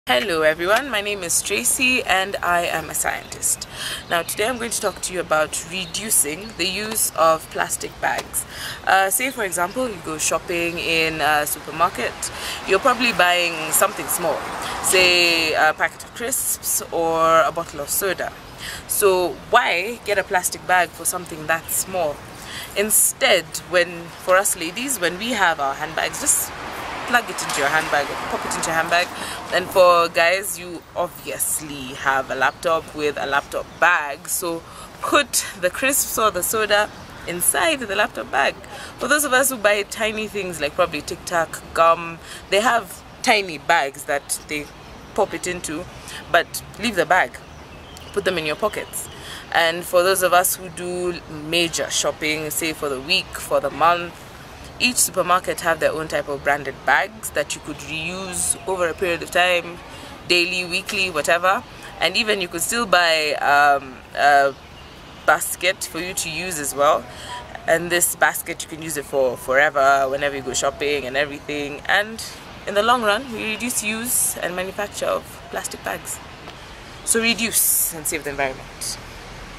Plastic Bags | RocKeSci
Tracey Shiundu, an industrial chemist, urges all to avoid use of polyethylene bags. Take a kiondo/bags to the supermarket or just use your pockets. These bags are the ones you see everywhere making the place look bad. Not to mention killing life.

RocKe Sci is a team of Kenyan earth science graduates with a passion  for sharing their knowledge with the world.